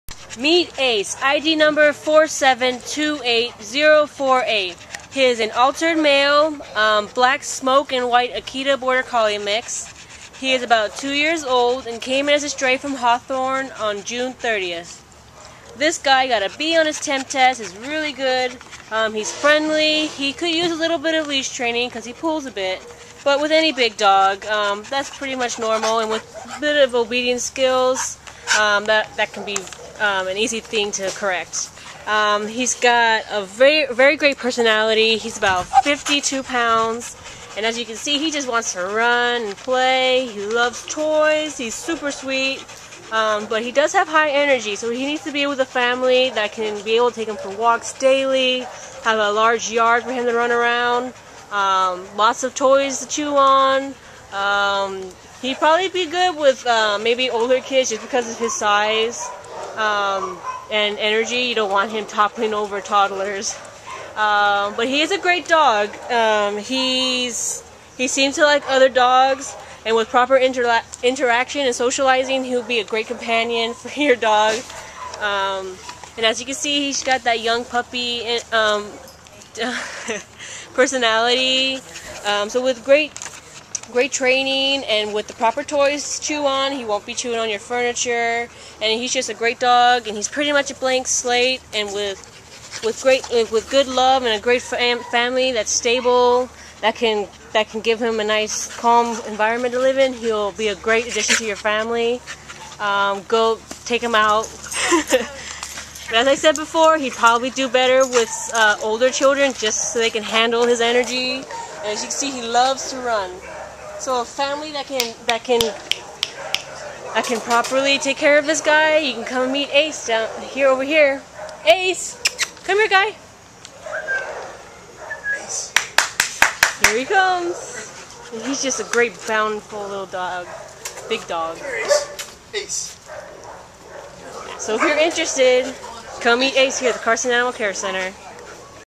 Ace A4728048
A4728048 Ace is a dynamic 2-year-old black smoke- and-white neutered male Akita Border Collie mix found in Hawthorne and brought to the Carson Animal Care Center on June 30th.  Ace scored a B on his temperament test!  Weighing 53 pounds, he is shy at first but otherwise friendly.  He does need leash and obedience training as he is a bigger dog with lots of higher energy.  He seems to like other dogs with proper introduction and socialization. He loves to run and play with toys and is great for an active family with older kids or someone looking for a running companion.  Ace is a great pet for anyone living in a private home with a yard.